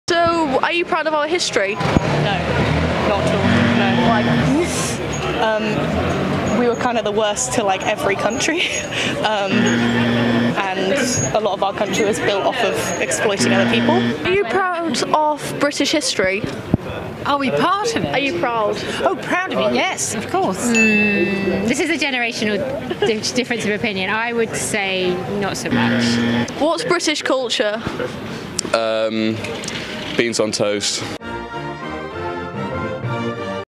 How much do Brits love their country?
Western nations have a habit of hating their own country. But what can we do about it? We must retrain them to have national pride.